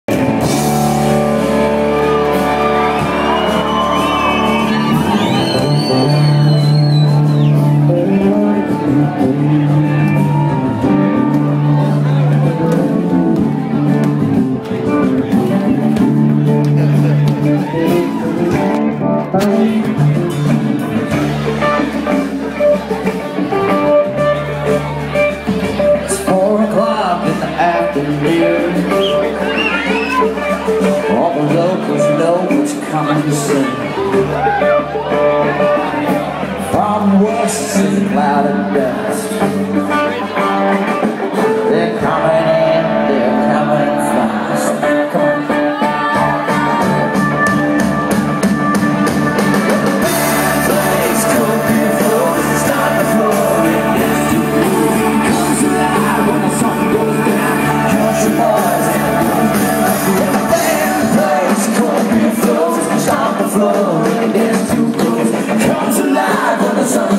Adam brand's birthday party
Adam brand's birthday party in Tamworth Country Music Festival 2011. The band in West Leagues club Tamworth